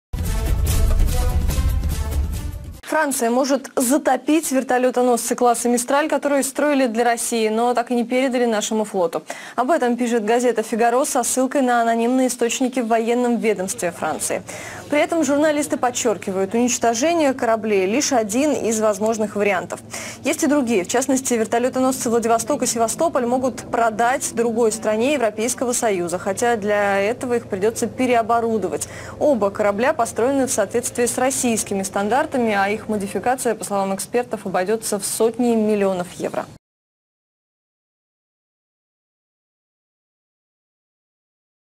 Франция готова затопить Мистрали в открытом море Новости России Украины Сегодня 06 05 20151
Франция готова затопить  Мистрали  в открытом море. У нас всегда свежие и актуальные новости и слухи из мира бизнеса, политики и развлечений.